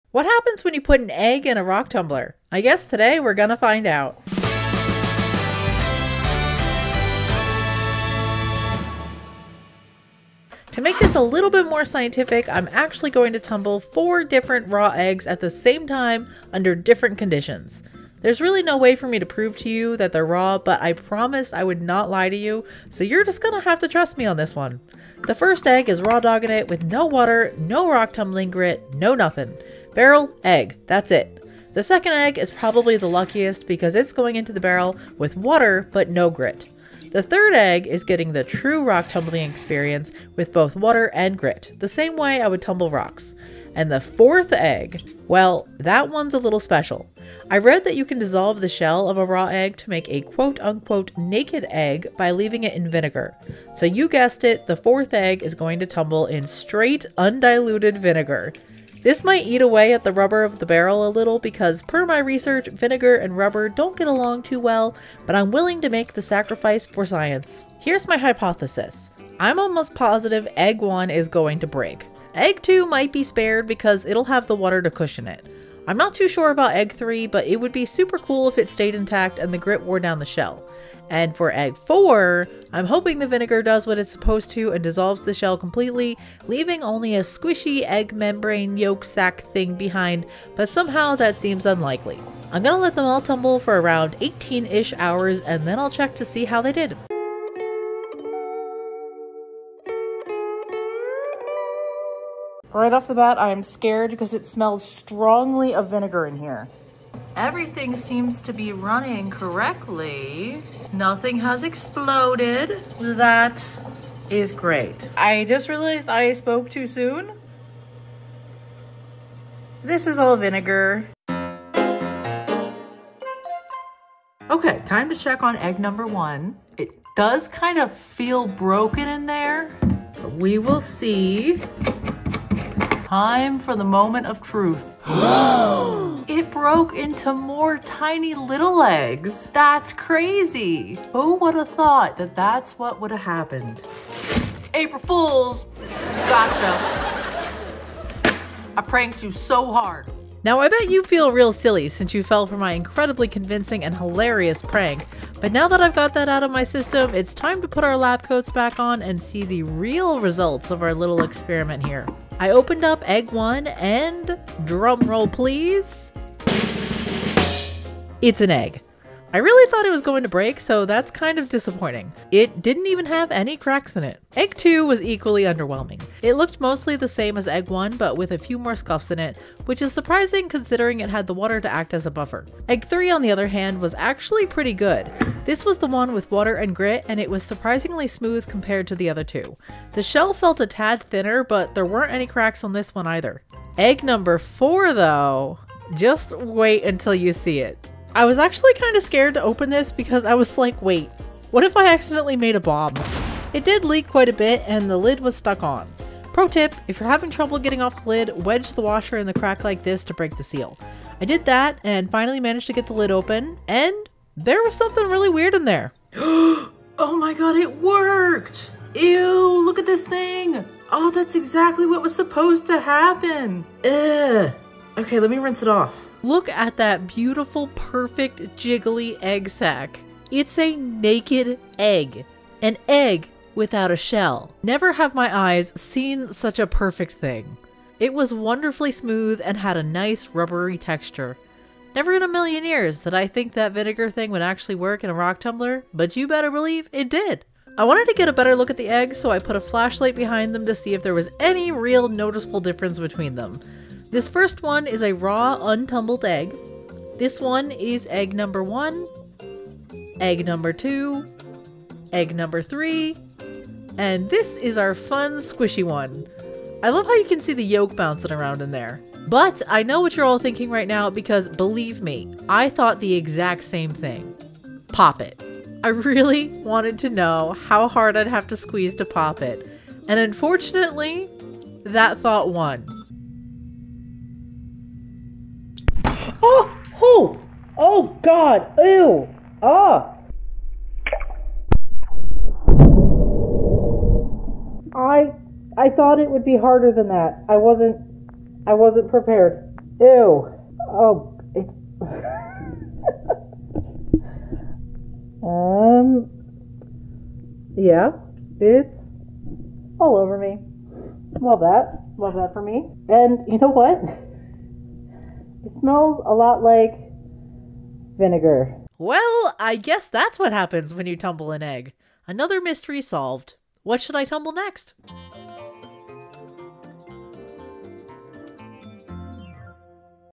What Happens When You Put an Egg in a Rock Tumbler?
Today we're going to find out what happens when you put an egg in a rock tumbler. Actually, four eggs. I really committed to the scientific process for this one. The results may *shock you*!

*Like cozy games with a sprinkle of chaos on top?

If you're reading this, comment "banana bread" down below!